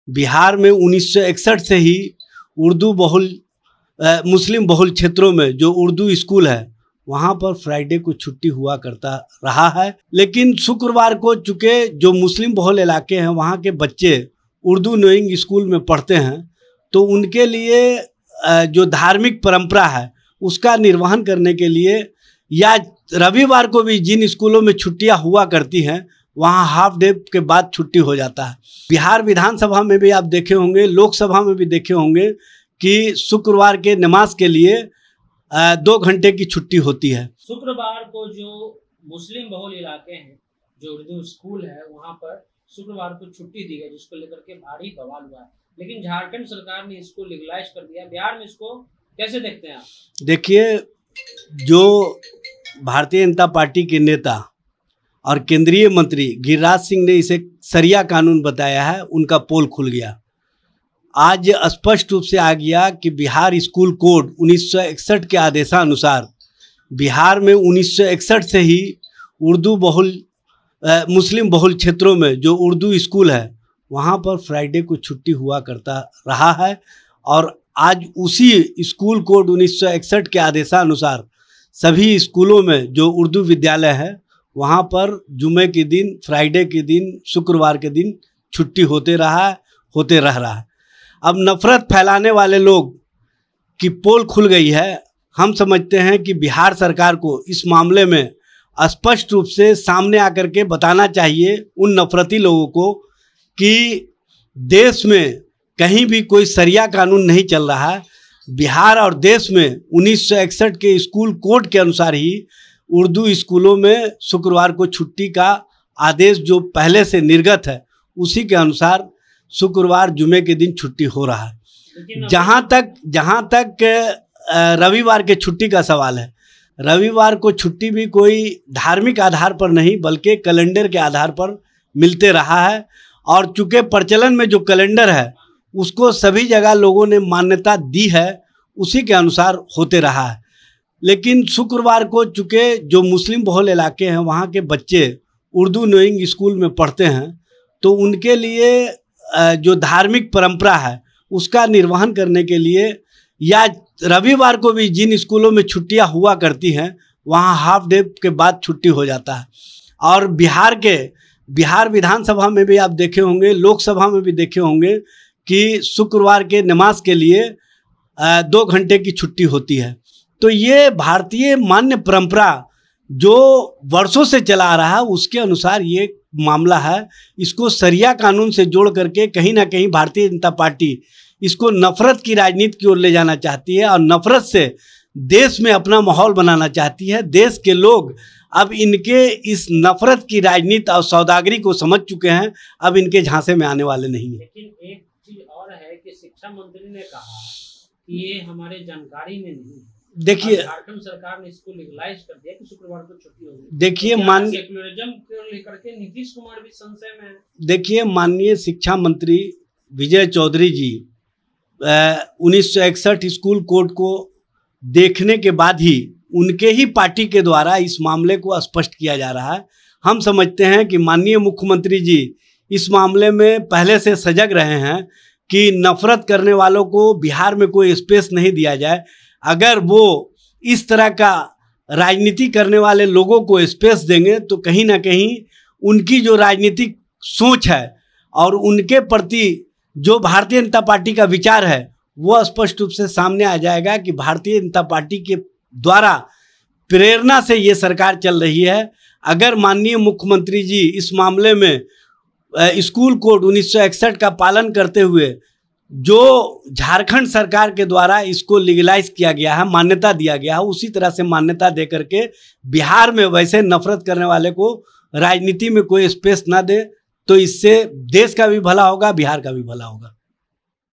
RJD नेता ने मुसलमान क्षेत्रों के बारे में क्या बताया? शुक्रवार को क्या जबरन छुट्टी कराई जाती है?
राजद के नेता ने मुसलमान बाहुल्य क्षेत्र में होने वाले शुक्रवार की छुट्टी पर क्या बोला? 1961 के बारे में क्या बताया? सुन लीजिए पूरी बात!